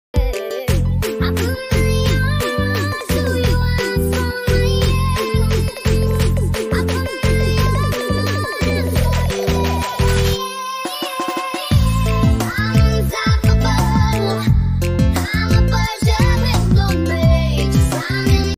Poor Panda's Luxury Suit Adventure
Join Panda on a fun-filled adventure as he dreams of wearing a luxurious suit! In this exciting short video, watch as Panda explores different ways to style himself in fancy outfits. From glamorous hats to sparkly shoes, Panda tries it all to look his best. Get ready for a delightful journey filled with laughter, creativity, and lots of fashion fun! Don't miss out on this adorable Panda's quest for the perfect outfit that will make him feel like a million bucks!